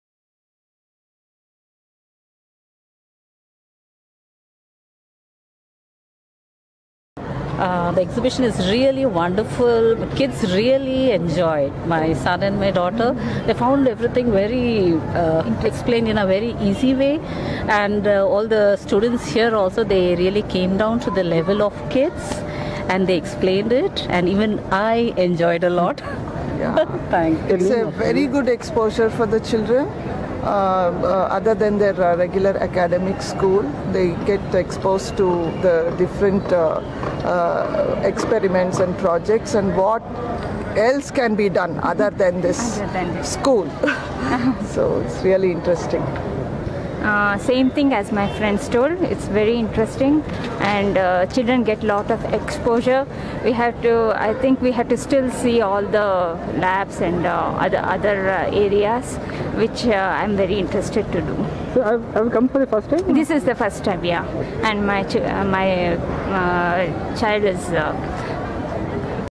Parents Speaking - IISC Open Day Event on March 1st, 2014
Parents speaking about their experience at IIsc,

to know more